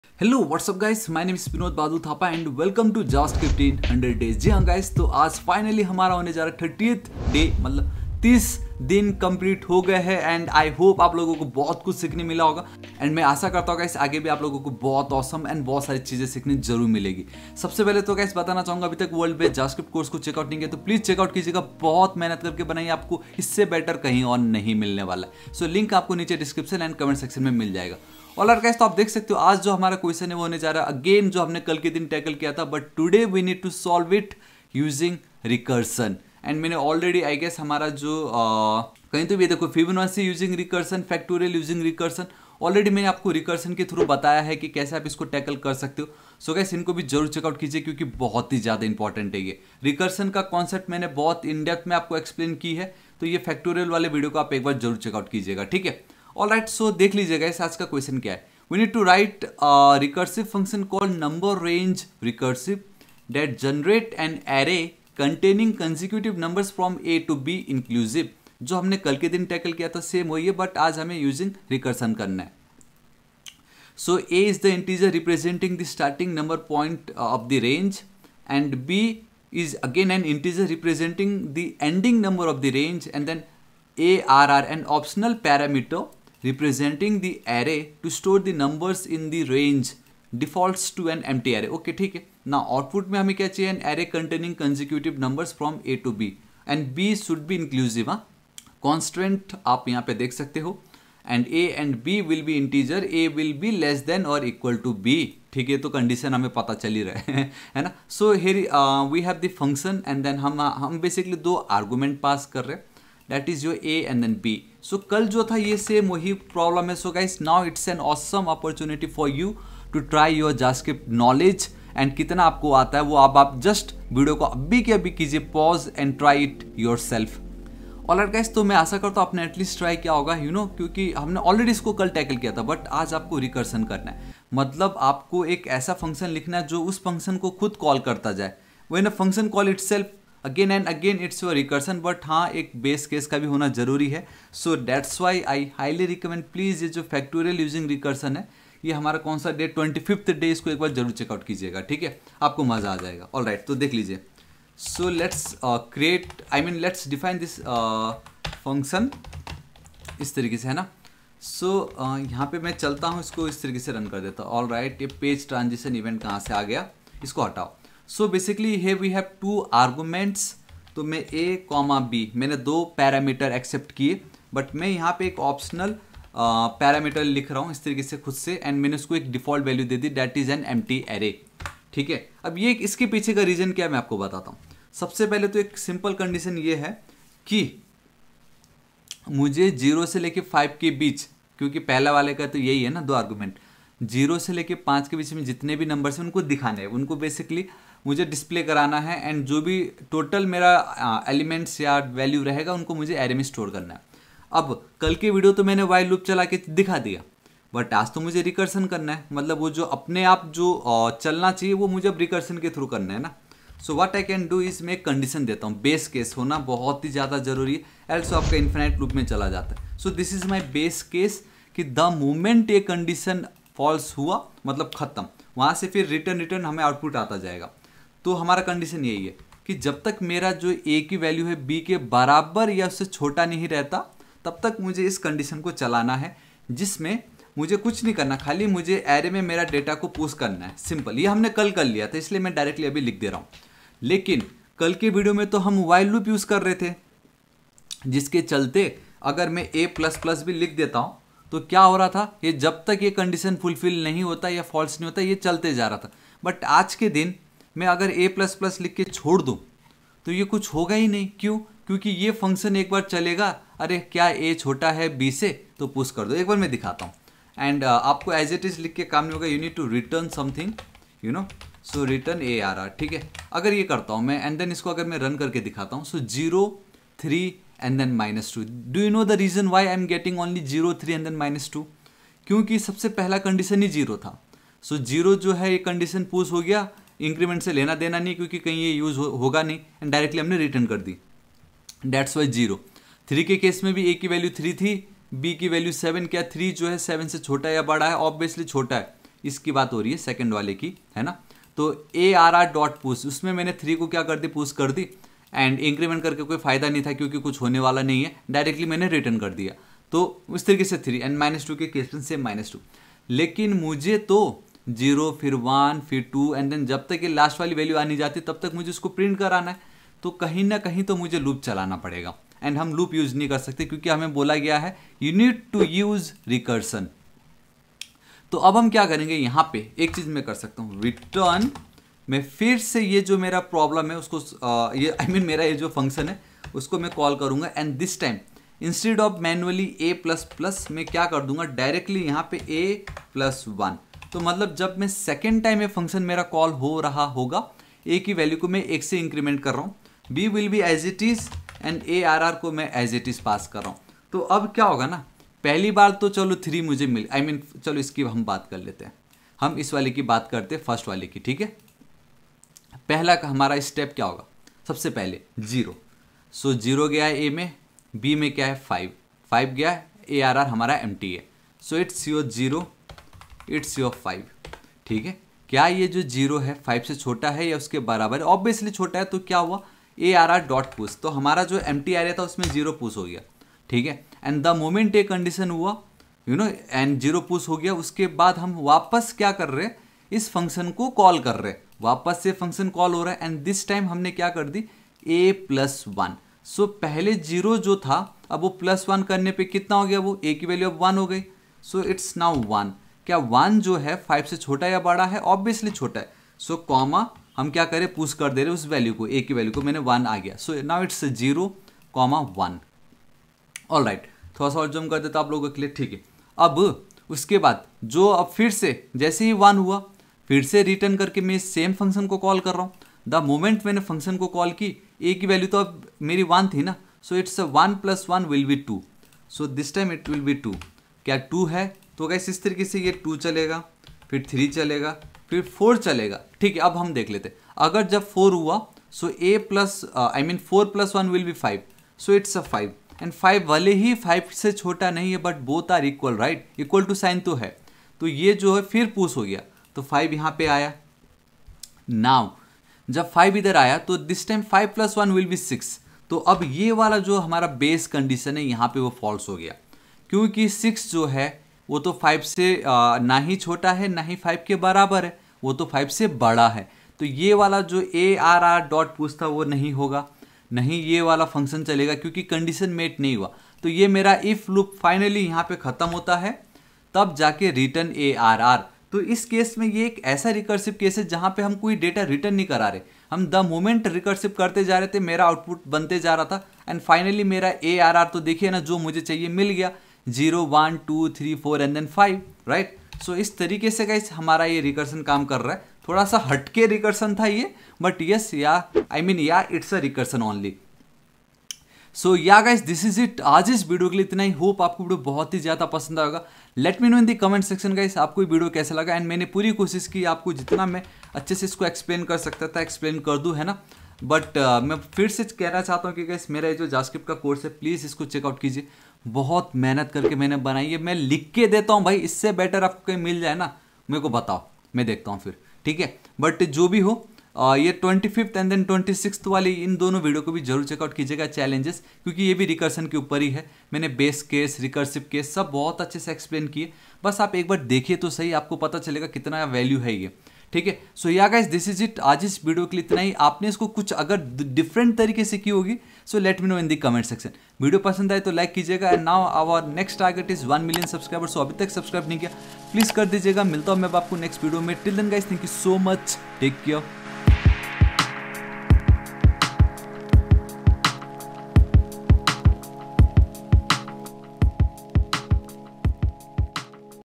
100 Days of JavaScript Coding Challenges || Day #30
Welcome, on a 100-day JavaScript journey with me! Join me as I tackle a new coding challenge every day, exclusively using JavaScript. Let's level up our skills together, one problem at a time.

📢Get your premium complete ecommerce website source code at no cost (Free)

⌚ TIMELINE ⌚
0:00 Introduction to JavaScript in 100 days
0:30 Understanding the questions
1:55 Pause the video and try it yourself
2:15 Solving the problem together
6:00 Understanding recursion in depth
8:20 Recommended video to watch
9:02 Conclusion and outro